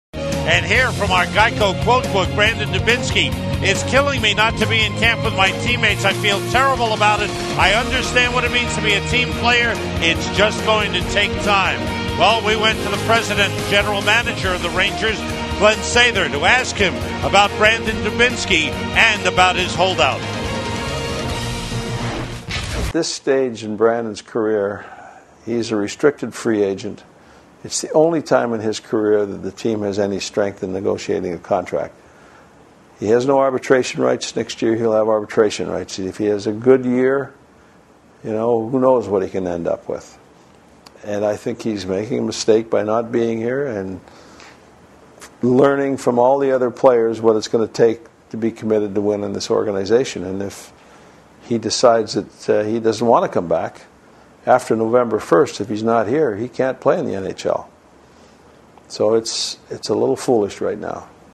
Glen Sather on Dubinsky Holdout
Rangers President and GM Glen Sather talks about the situation with Brandon Dubinsky.